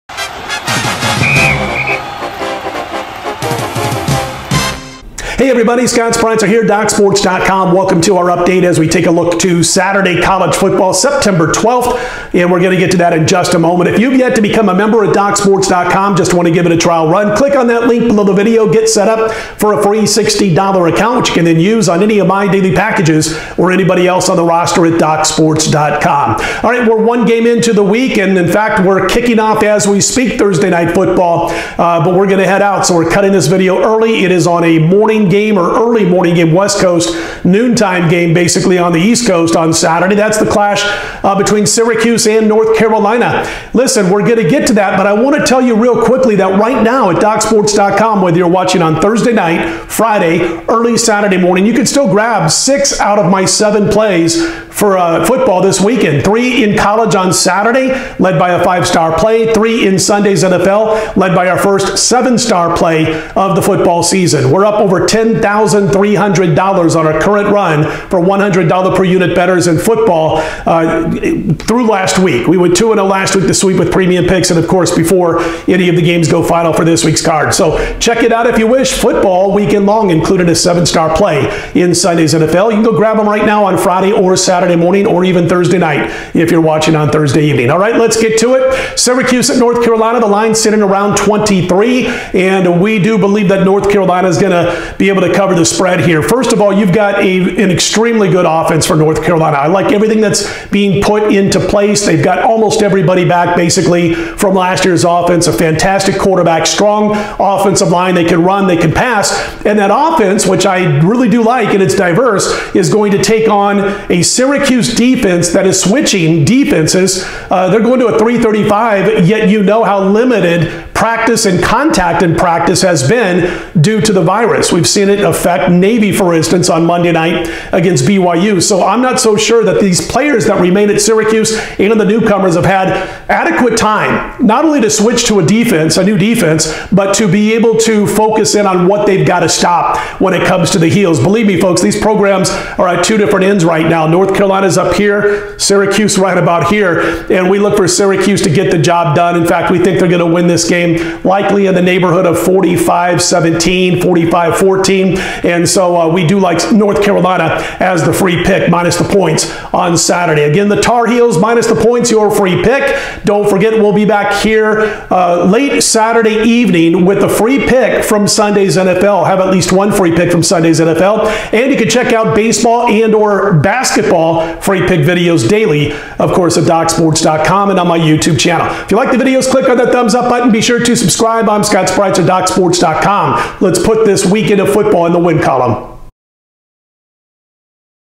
Syracuse Orange vs. North Carolina Tar Heels 9/12/2020 College Football Picks, Predictions, Tips
North Carolina Tar Heels vs Syracuse Orange College Football Pick and Prediction 9/12/2020. The Syracuse Orange travel to Chapel Hill, NC to face the North Carolina Tar Heels at 12:00PM EST at Kenan Memorial.

Doc's Sports provides College Football picks, predictions, tips and college football odds on this matchup of Syracuse Orange vs North Carolina Tar Heels.